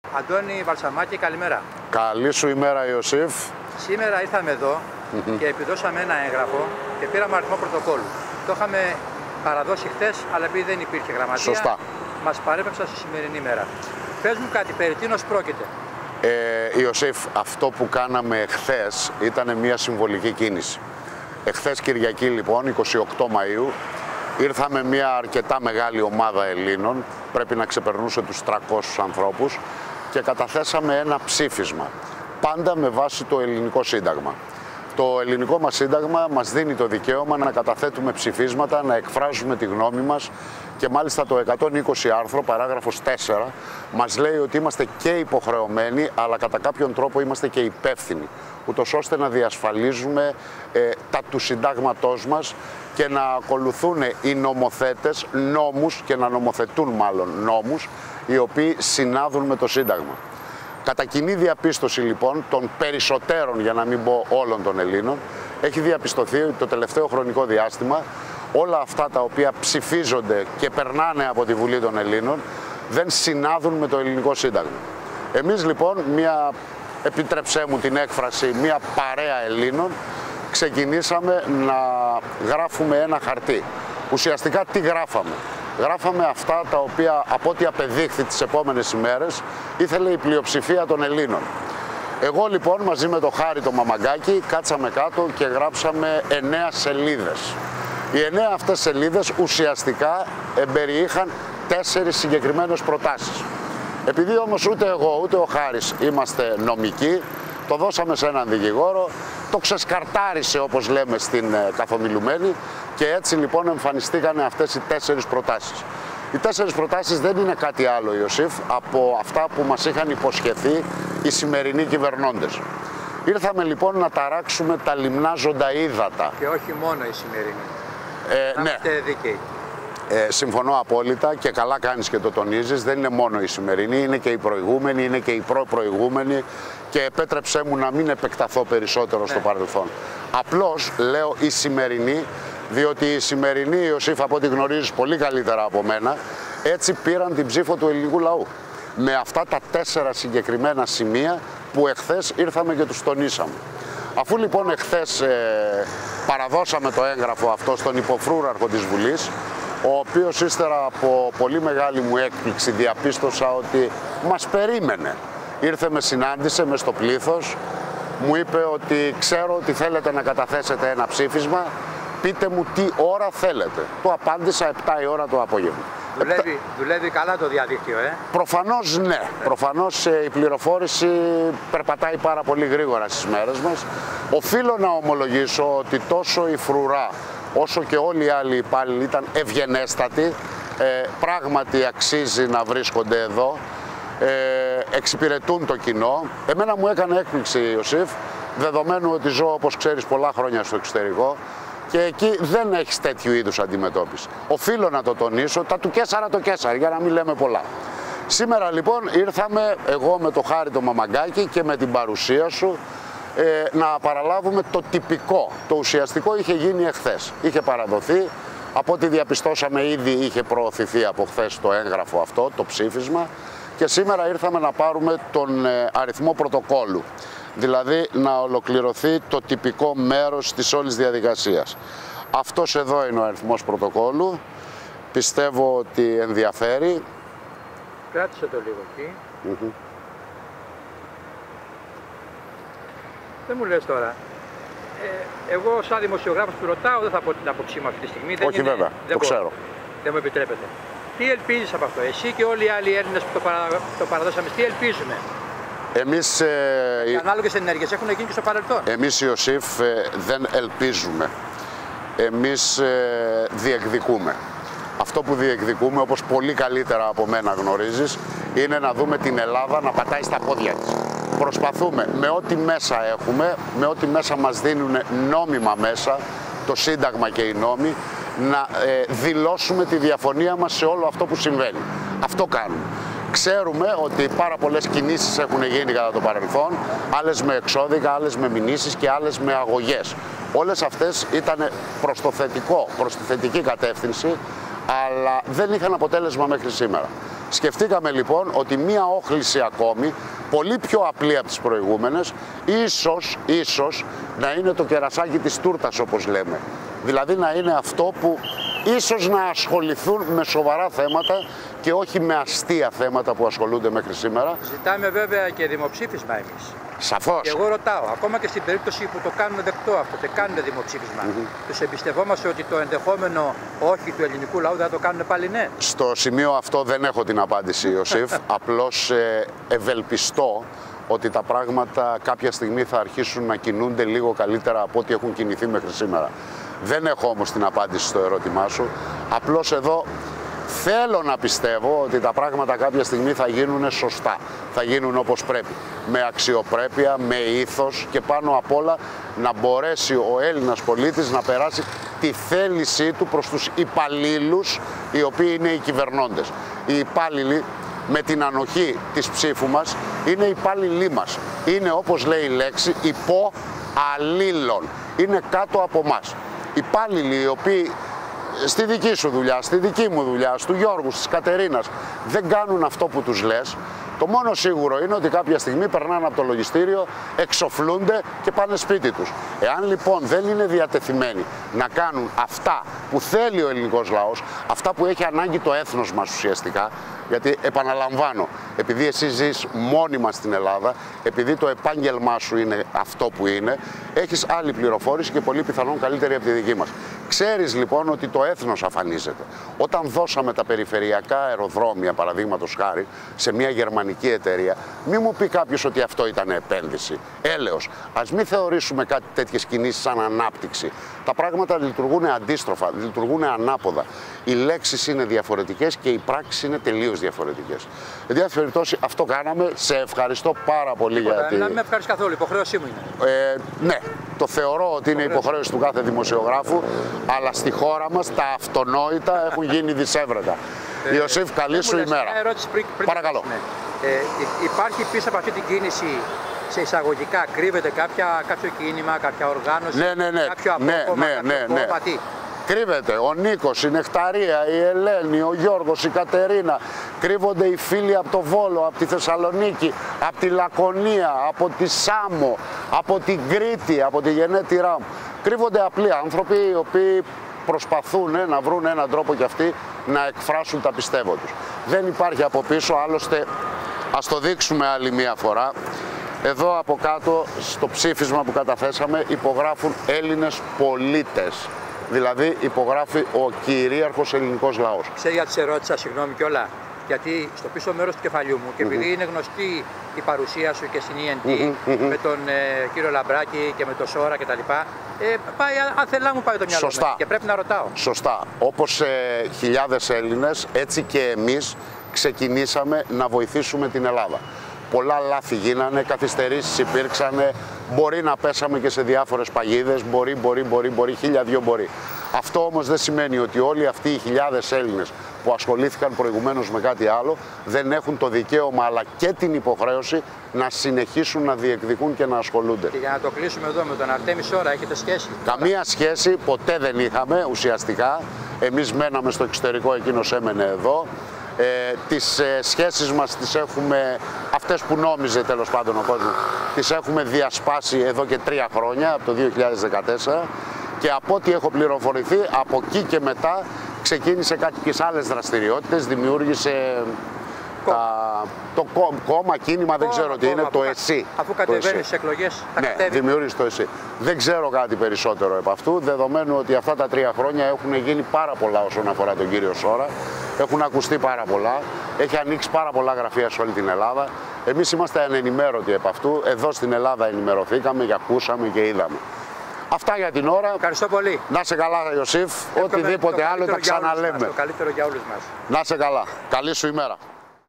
ΕΠΙΔΟΣΗ ΨΗΦΙΣΜΑΤΟΣ ΕΛΛΗΝΩΝ ΠΟΛΙΤΩΝ ΣΤΗ ΒΟΥΛΗ
Άλλη μια πρωτοβουλία Ελλήνων πολιτών, άλλο ένα ψήφισμα κατετέθη χθες στη Βουλή με πρωτοβουλία των Αντώνη Βαλσαμάκη και Χάρη Μαμαγκάκη. Μαζί τους η Φλώρα Ασημακοπούλου και ο υπογράφων αυτές τις γραμμές, (απρόβλεπτος) δημοσιογράφος Ιωσήφ Παπαδόπουλος. Στην προκαθορισμένη ώρα (6 μ.μ.) 100 περίπου πολίτες συγκεντρώθηκαν στον χώρο μπροστά από το Μνημείο του Αγνώστου Στρατιώτη ενώ, την ίδια στιγμή, με μεγάλη έκπληξη είδαμε τον επικεφαλής της αστυνομικής φρουράς να πλησιάζει και να ζητάει να μάθει με ποιο τρόπο σκεπτόμαστε να επιδώσουμε το ψήφισμα! Σημάδι πως τα παπαγαλάκια του διαδικτύου είχαν και πάλι δουλέψει καλά... Περισσότερα